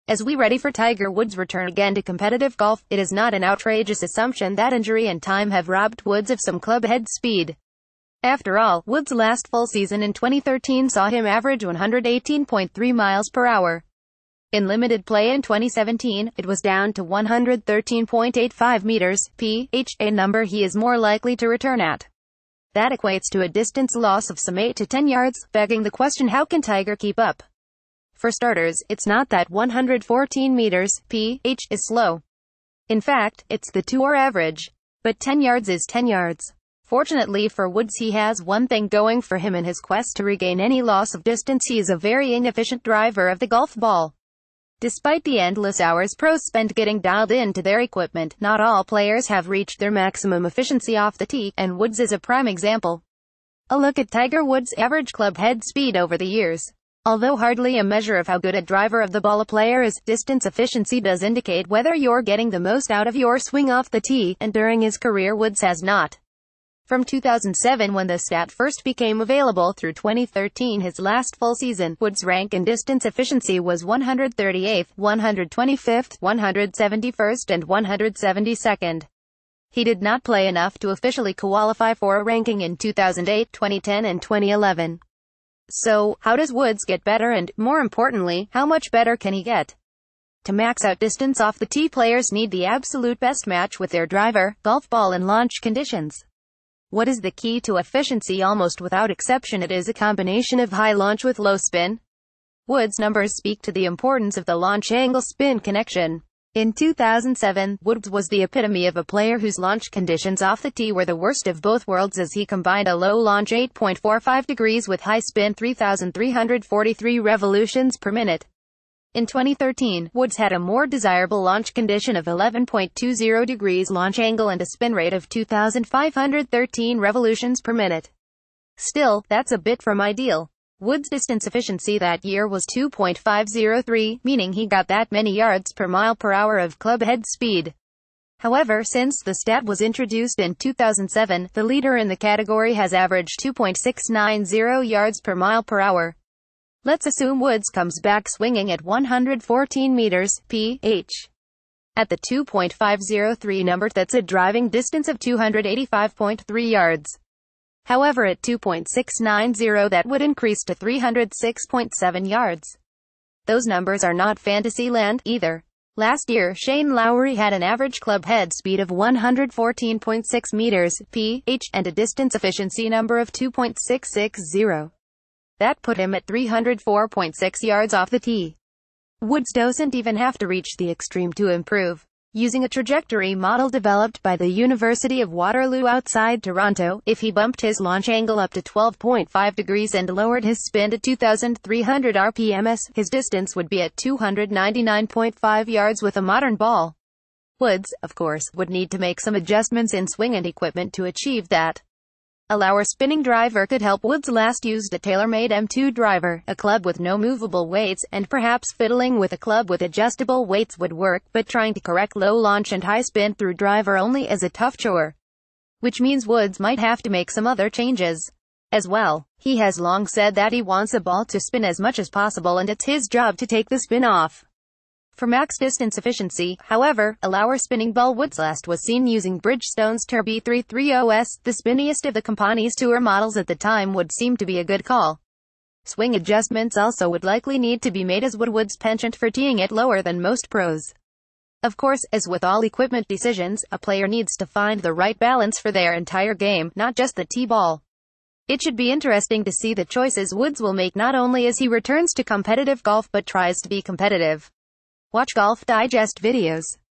An important question for tiger woods' return: can he take better advantage of modern equipment?.
As we ready for Tiger Woods’ return again to compe...